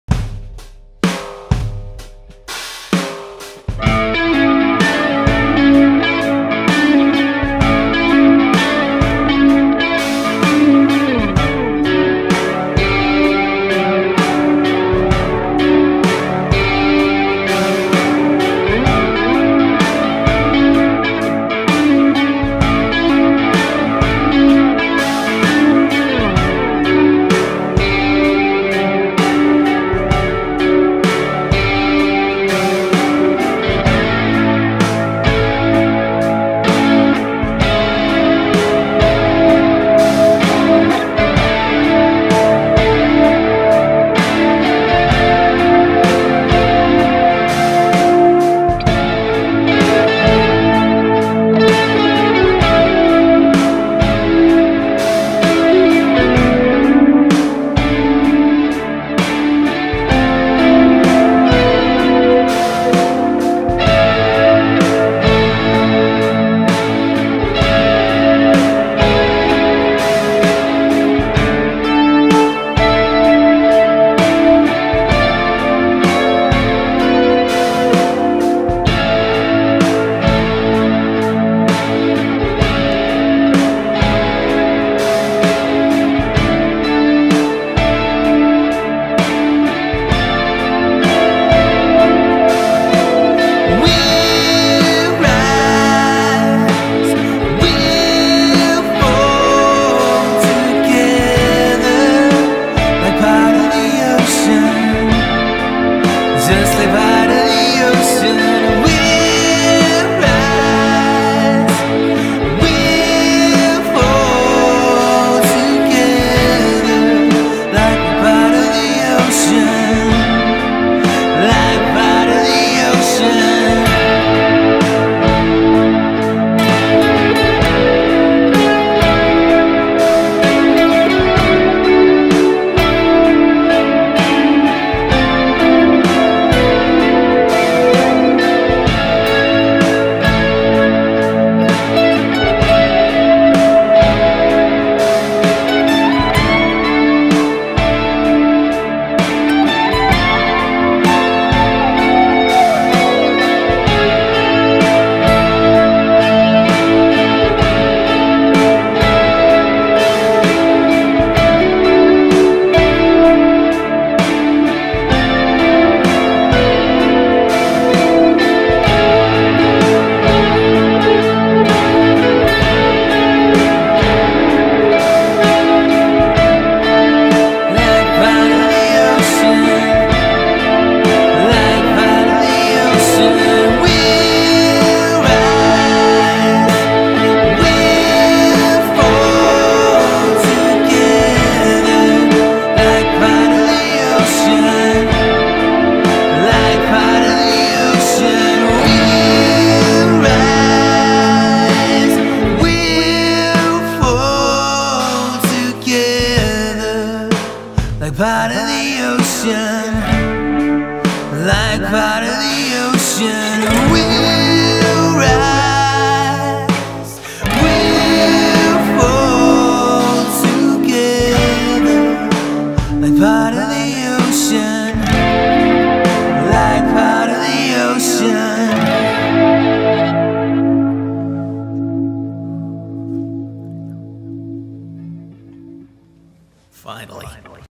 Live Guitar and Singing | Maunster's Jamuary2019 | EP25
I miss playing guitar. So I decided to fix that. And, wow. I should practice more.